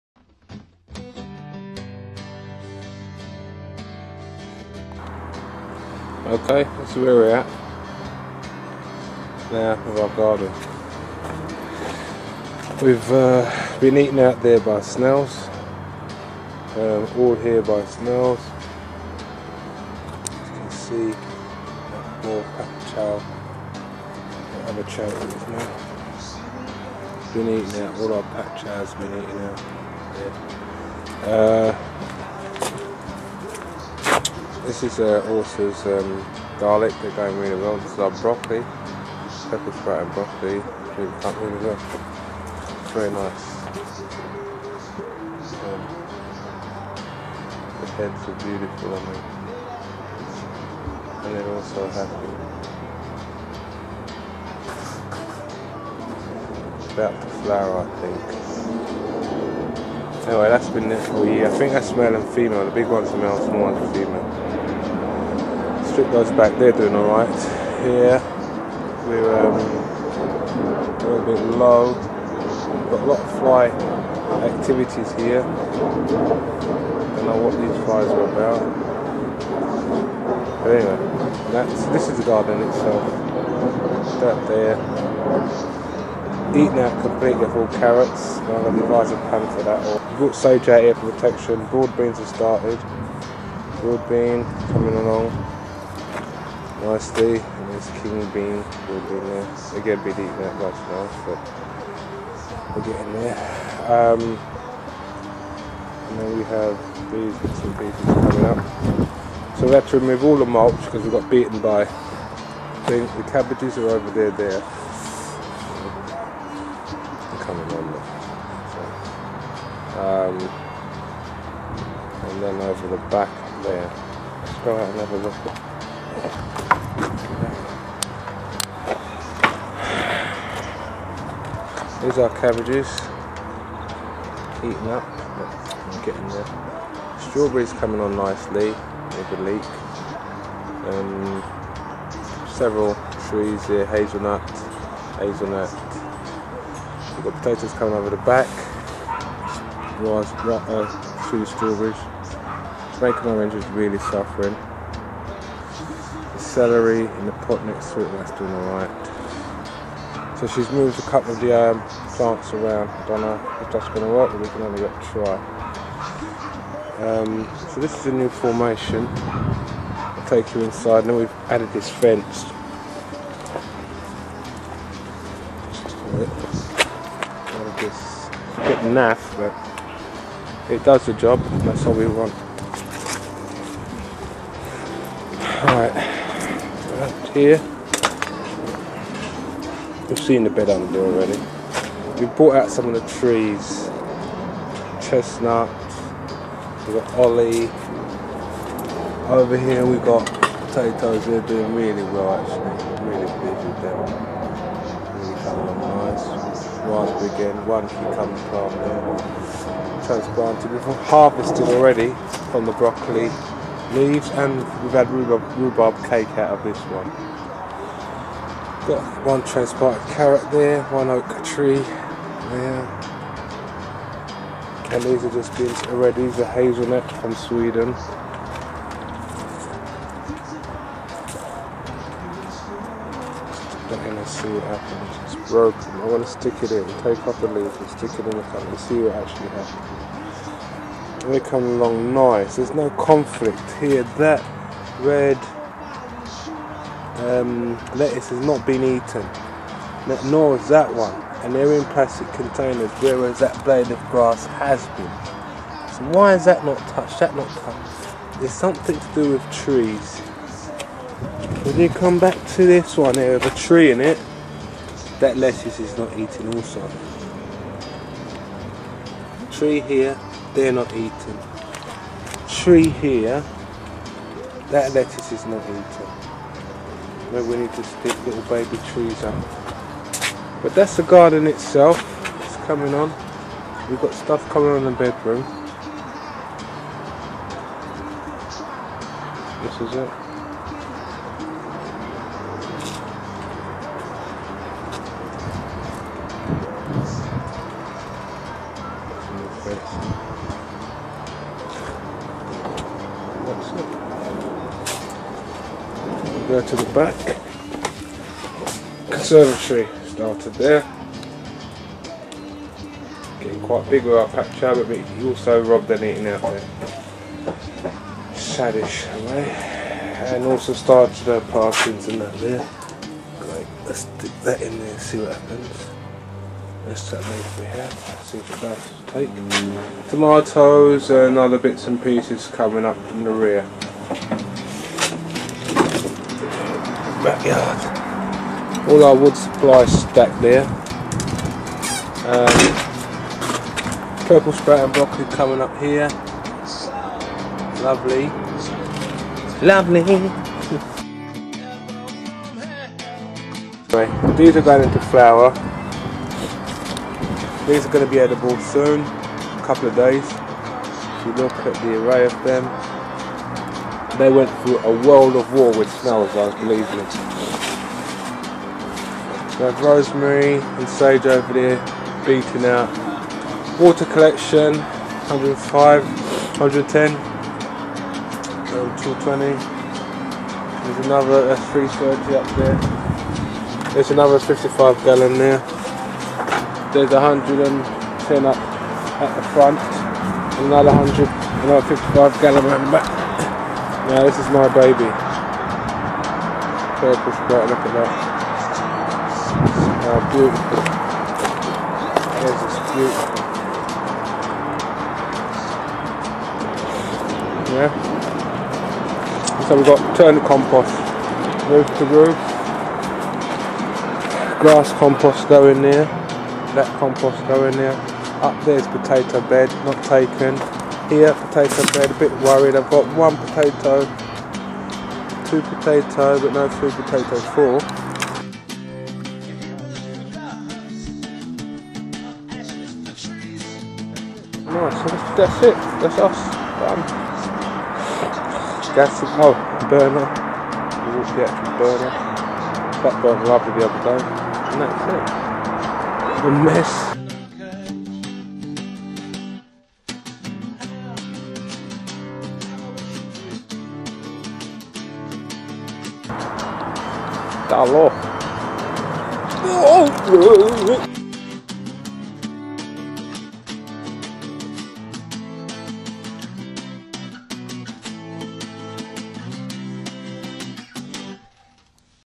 Growing your health from scraps part 5 slugs and snails
Into first stage of planting and the fight had with snails in the beginning ,this video goes on to show the back and the piles of composts we accumulate over the years basically giving nothing away to our local bin men ,,less work for them and more nutrients for us  ,. This was the first major planting of the beds since construction,again much had to be learned by mistakes  but glad they had been made in the end you get paid with good health and nutrients enjoy and give some kind feed back God bless